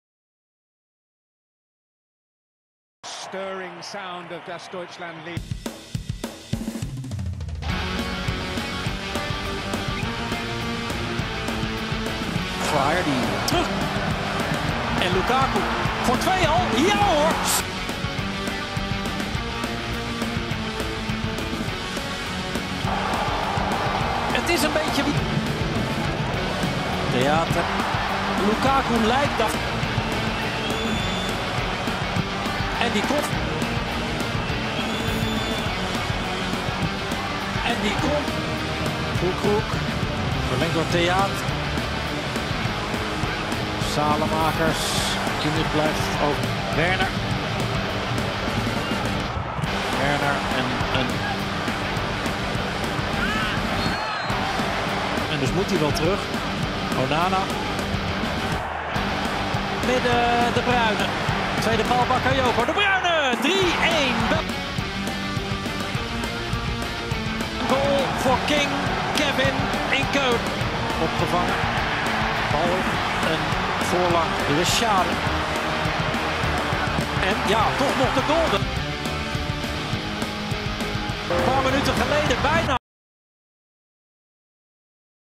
Jerman vs Belgia 2-3 | International Friendlies 2023 | Highligts
Jerman vs Belgia
Jerman vs Belgia

* Germany
  Goals
  Fullkrug (44')
  Gnabry (87')

* Belgium
  Goals
  Carrasco (6')
  Lukaku (9')
  De Bruyne (78')